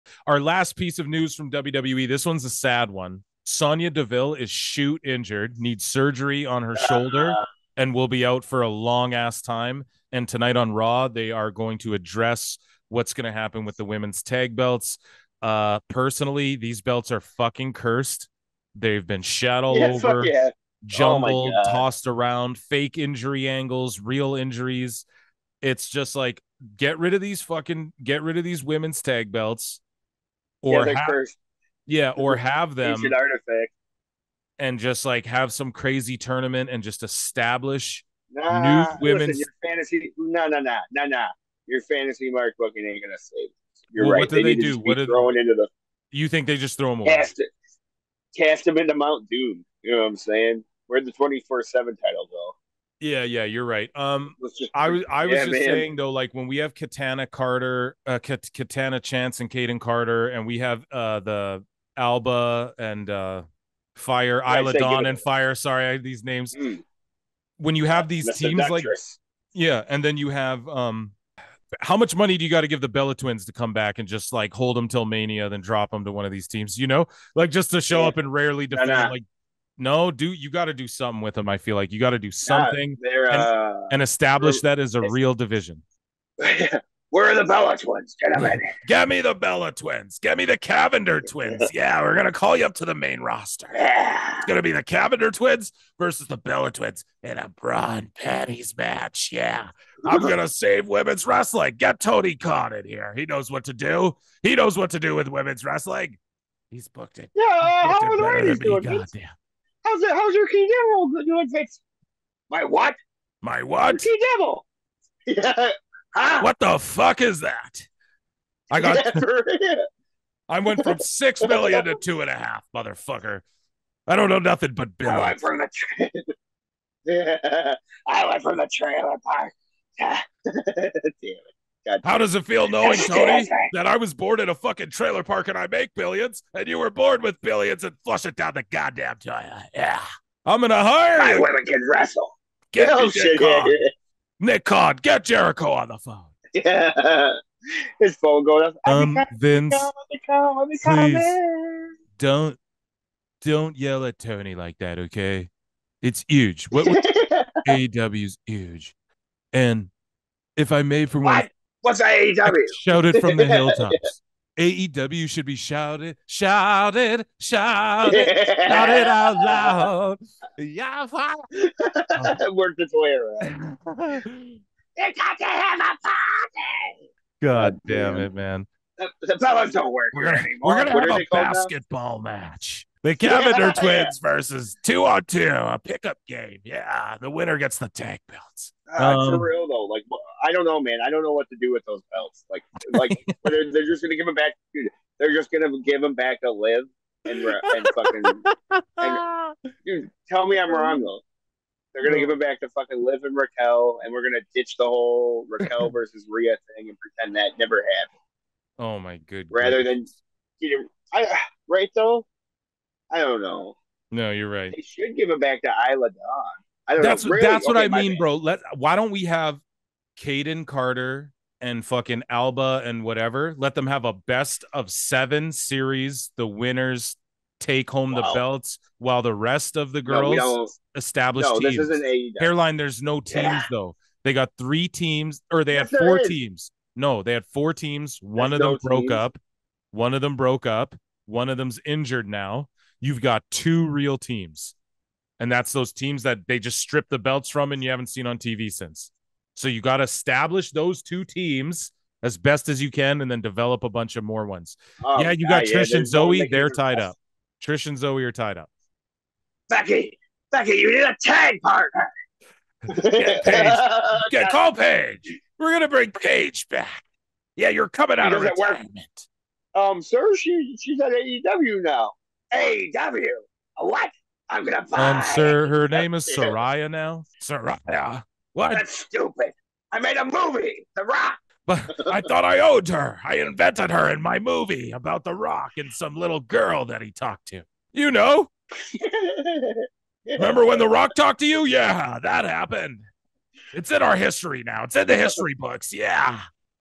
Sonya Deville INJURED! WWE Women's Tag Titles Are CURSED?
In this clip taken from PWT Podcast 20, Hairline & Phil talk about how Sonya Deville is injured and how the WWE Women's Tag Team Titles are cursed. Check out full episodes below!👇